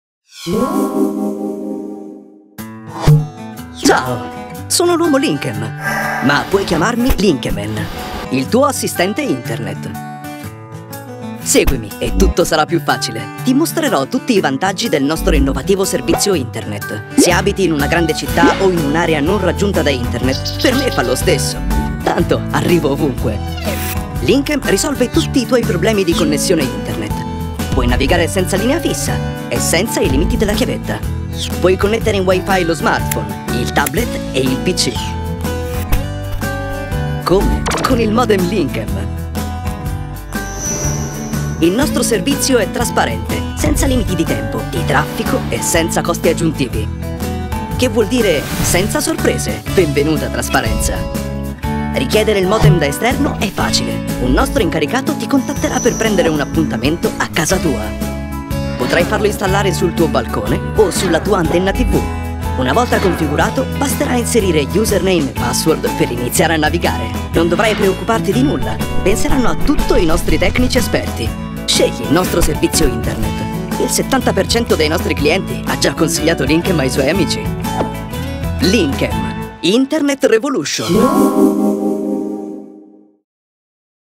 Modem da esterno Linkem
Scopri il modem da esterno Linkem, per navigare sempre senza limiti!